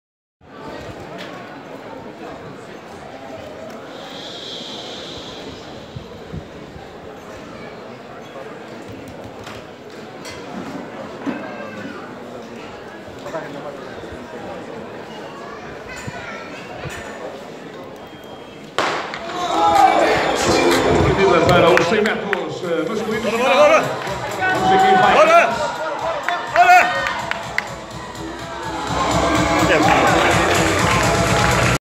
100m campeonatos Portugal
10.10 + 2,9 Diogo Antunes e melhor Final de 100 metros em Portugal,  Vagos, 10 de Junho de 2017